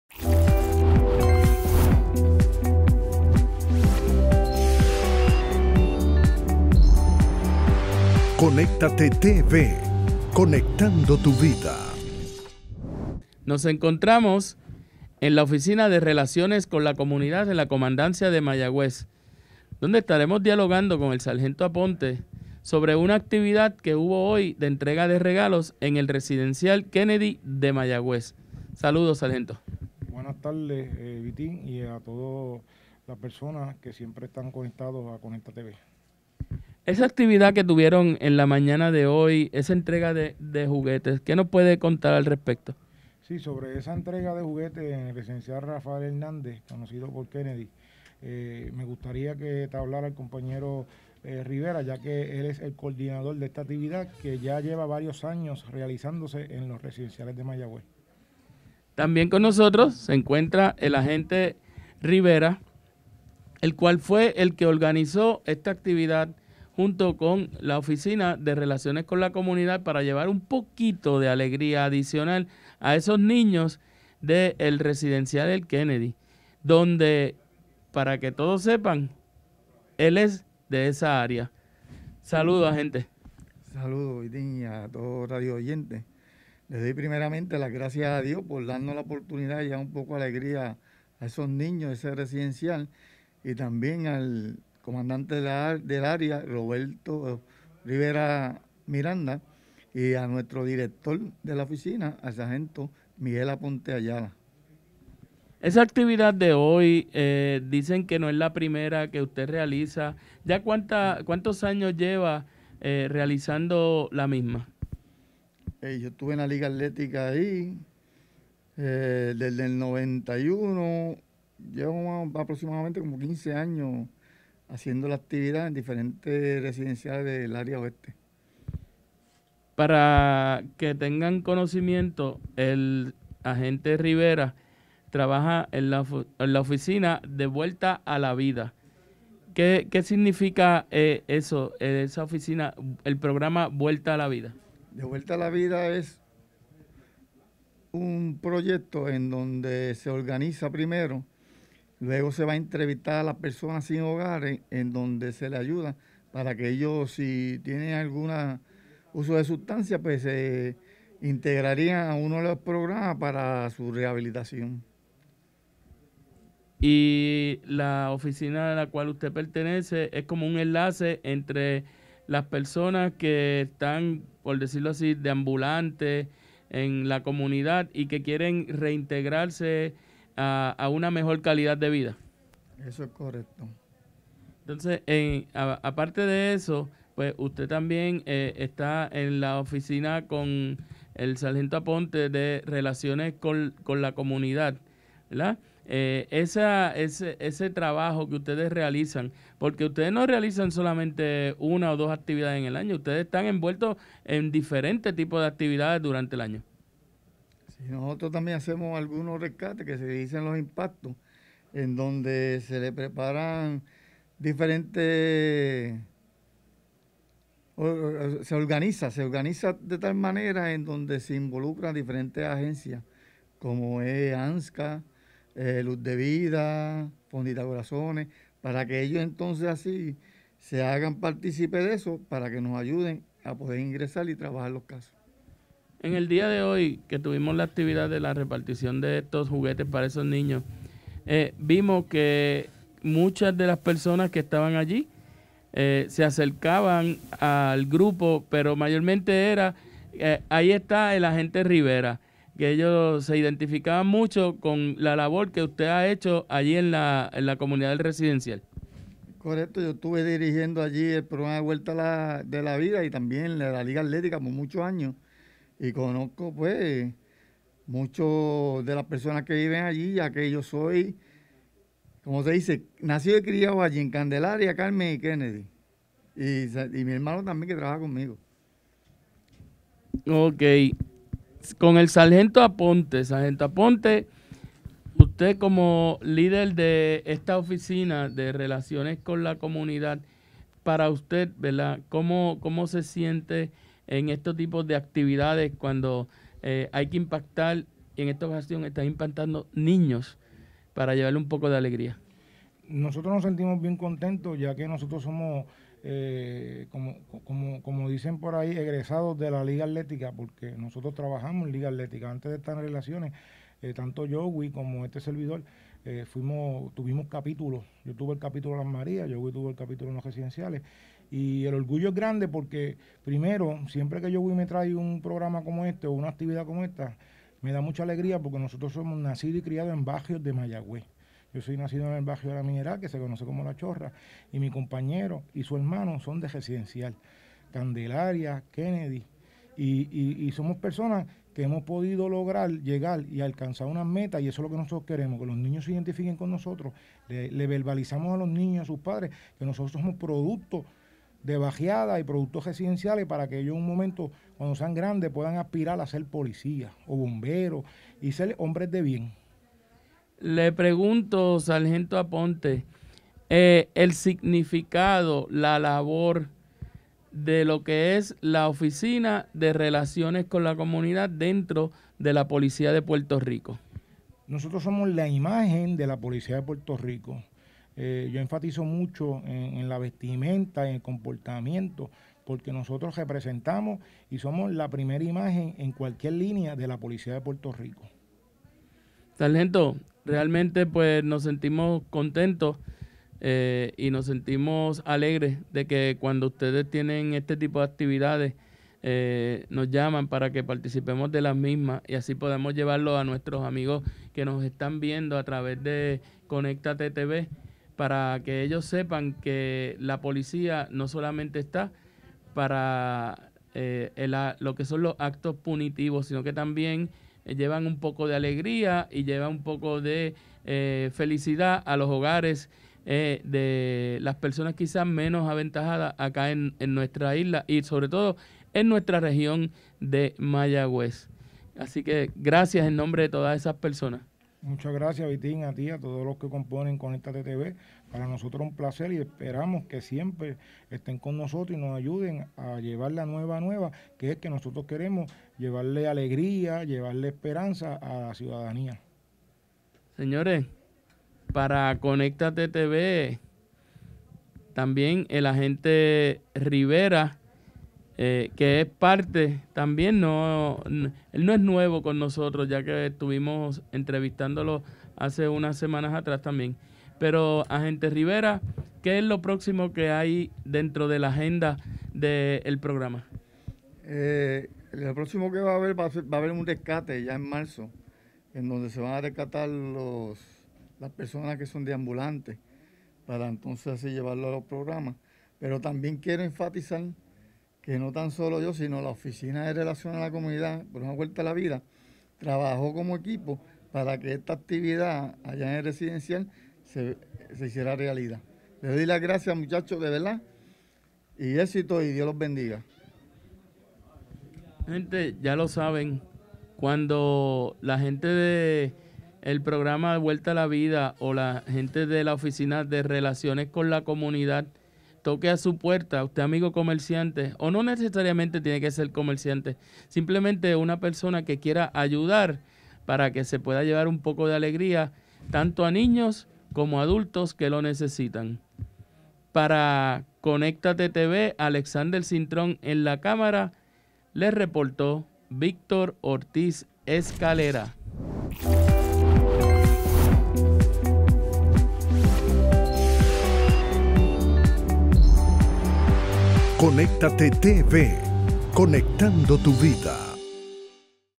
Conéctate TV Noticias Entrevista al Sgto Aponte y el Agente Rivera entrega de regalos en Kennedy
Conéctate TV Noticias - Víctor Ortíz entrevista al Sgto. Aponte y el Agente Rivera sobre la entrega de regalos en el Res. Rafael Hernández(Kennedy) de Mayagüez